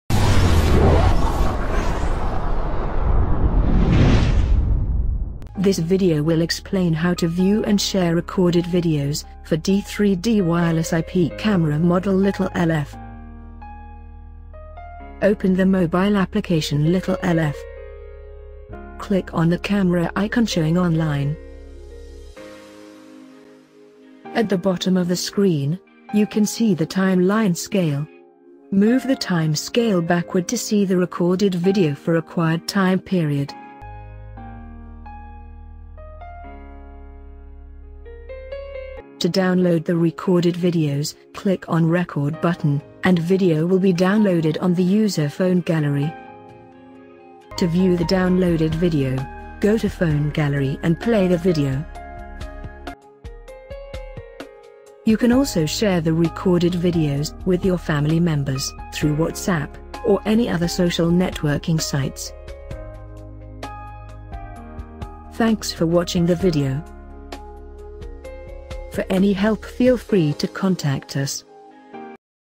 How to view or download recorded videos in D3D smart wireless HD IP camera Model Number-LittleLF
This video will explain how to view/download or share recorded videos in D3D wireless HD IP camera Model – LF-P1S. This Process is very easy and takes only 5 minutes to complete it.
D3D Wireless Smart IP camera is provided with all accessories and fixing required to set up the camera. During configuration please make sure of followings:
1. Follow the step by step process.
2. There should be no special characters (£$%&@...) in your Wi-Fi Password.